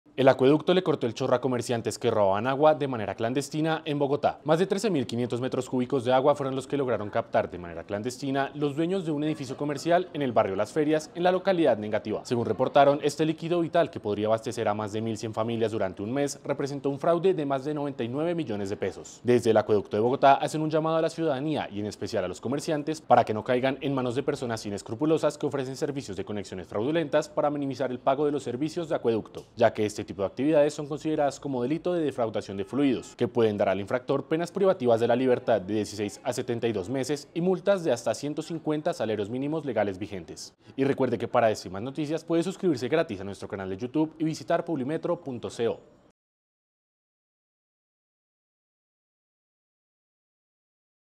Acueducto descubrió locales comerciales que robaban agua en Engativá de manera clandestina
En la localidad de Engativá, en el sector del barrio Las Ferias, la Empresa de Acueducto y Alcantarillado de Bogotá (EAAB), con apoyo de la Policía Nacional, realizó un operativo en el que se identificó una conexión clandestina que logró captar de manera irregular más de 13.500 metros cúbicos de agua.

La acción, que tardó más de 8 horas, se llevó a cabo en una propiedad de tres pisos que albergaba ocho locales comerciales de venta de alimentos, una dulcería, un local con servicios de ortopedia, una licorera, una bodega y dos apartamentos.

Con el fin de encontrar la conexión ilegal, se llevaron a cabo trabajos con maquinaria especializada para realizar excavaciones en vía pública e identificar una compleja red alterna que configuraba la conexión, que, además, avanzaba por la fachada del inmueble hasta la azotea, lo que hacía compleja su ubicación.